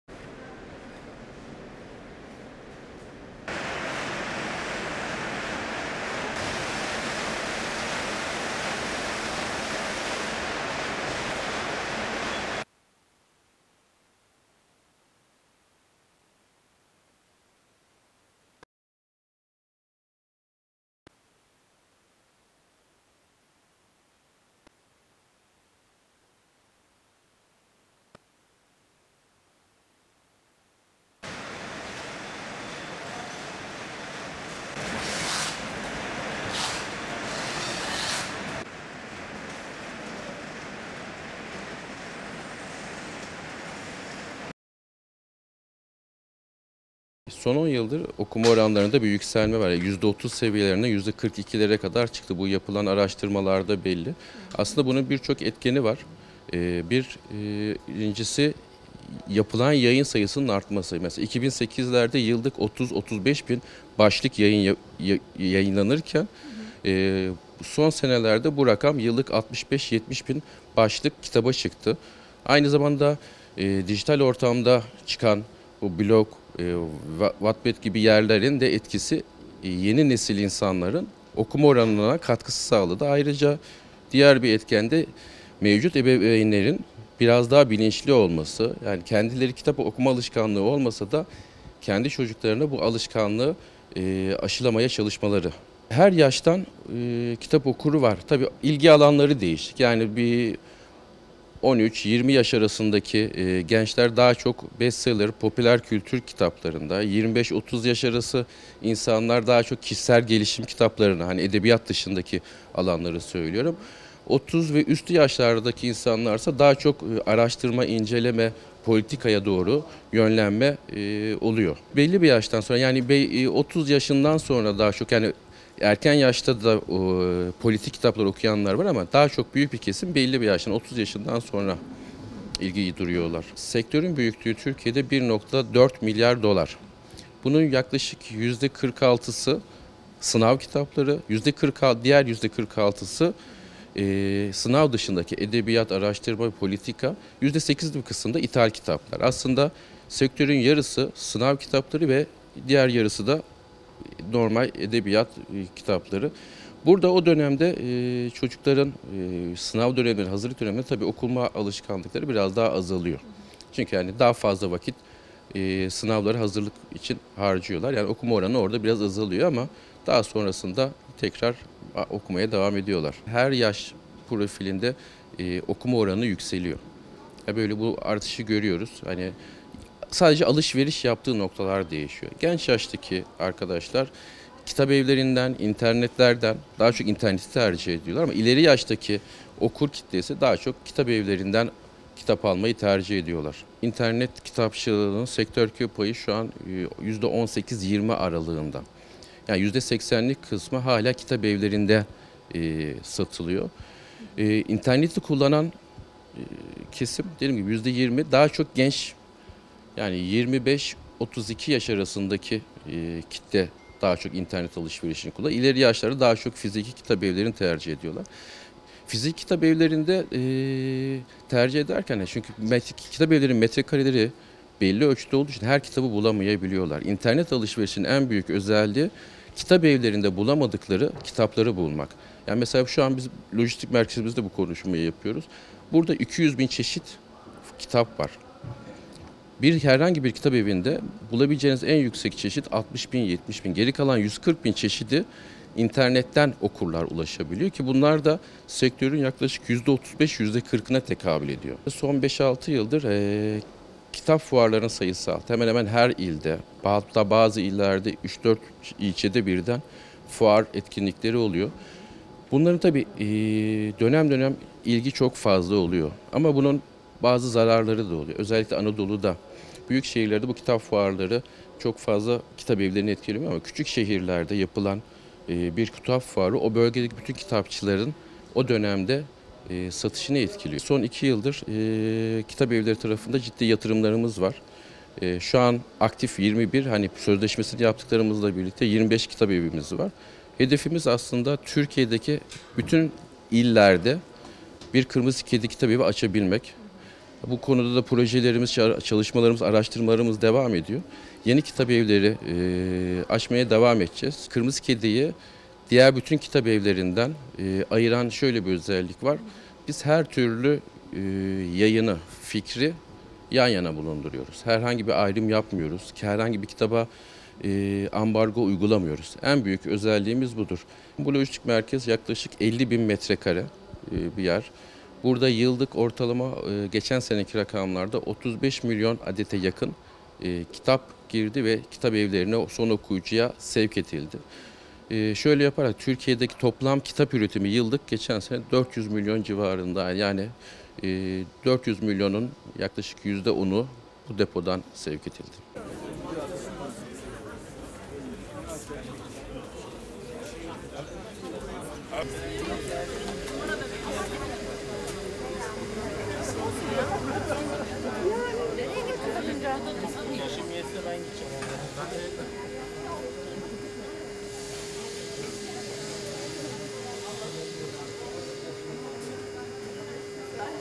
Kitap okuma oranı 10 yılda yüzde 10 arttı
Türkiye'nin kitap okuma oranında son 10 yılda artış olduğunu belirten Emek Kitap Genel Müdürü Murat Bahadır, "Araştırmalara göre, okuma oranı son 10 yılda yüzde 30'dan 40'lara kadar çıktı" dedi. Bahadır, bu artışın nedenlerini ve yaşa göre tercih edilen kitap türlerini açıkladı.

Türkiye'nin en büyük kitap dağıtım şirketlerinden olan ve Babil, Eganba, OdaKitap gibi online kitap satış sayfalarını ve Kırmızı Kedi Kitabevlerini de bünyesinde barındıran Emek Kitap’ın Genel Müdürü Murat Bahadır, Türkiye’deki kitap satışı ve okuma oranları üzerine bilgi verdi. Türkiye'nin yıllık toplam kitap üretiminin 400 milyon civarında olduğunu belirten Bahadır, "400 milyonun yaklaşık yüzde 10'u bu depodan gidiyor. Avrupa ülkelerinden çok daha gerideyiz ama rakamlar her yıl artıyor. Devletin okuma alışkanlığına katkısı olması gerekiyor" dedi.

"OKUMA ORANI SINAV DÖNEMİ DÜŞÜYOR"

Türkiye'de kitap sektörünün büyüklüğünün 1.4 milyar dolar olduğunu belirten Murat Bahadır, bu rakamın yüzde 46'sını sınava hazırlık, diğer yüzde 46'sını edebiyat, araştırma, politika, yüzde 8'ini ise ithal kitapların oluşturduğunu ifade etti. Çocukların okuma alışkanlıklarının sınav döneminde azaldığını vurgulayan Murat Bahadır, sınav dönemleri sonrası okuma oranlarında tekrar bir artış gözlemlediklerini söyledi. Bahadır, Türkiye’nin kitap okuma oranında son yıllarda sanılanın aksine artış olduğunu ifade etti.

KİTAP OKUMA ORANI YÜZDE 30’DAN 40’A ÇIKTI

Yapılan araştırmalara dikkat çeken Murat Bahadır, "Okuma oranı yüzde 30'dan 40'lara kadar çıkmış durumda. Bunun birçok etkeni var. Birinci sebep, yapılan yayın sayısındaki artış. 2008'lerde yılda 30 ila 35 bin başlık yayınlanırken son yıllarda bu rakam yıllık 65-70 bin başlık kitaba çıktı. Aynı zamanda dijital platformlar da (blog, wattpad gibi) yeni nesil insanların okuma oranına katkı sağladı. Bunun yanı sıra ebeveynler artık daha bilinçli. Ebeveynler kendilerinde kitap okuma alışkanlığı olmasa bile çocuklarına bu alışkanlığı aşılamaya çalışıyorlar" değerlendirmesinde bulundu.

GENÇ OKURUN TERCİHİ KİŞİSEL GELİŞİM KİTAPLARI

Her yaştan kitap okurunun olduğunun altını çizen Murat Bahadır, okurların ilgi alanında yaşa göre farklılıklar olduğunu söyledi. Bahadır, "13-20 yaş arasındaki gençler daha çok bestseller (çok satan kitaplar) ve popüler kültür kitaplarını tercih ediyorken, 25-30 yaş arası ise kişisel gelişime yöneliyor. 30 yaş ve üstünün tercihi ise araştırma-inceleme ve politika kitapları oluyor. Siyasi kitaplar 30 yaştan sonra daha çok tercih ediliyor. Okuma oranı her yaş profilinde yükseliyor. Artışı görüyoruz, sadece insanların alışveriş yaptıkları noktalar değişiyor. Genç yaş grubu kitap alışverişinde daha çok interneti tercih ederken ileri yaş okur kitlesi daha çok kitabevlerinden kitap alıyor" diye konuştu.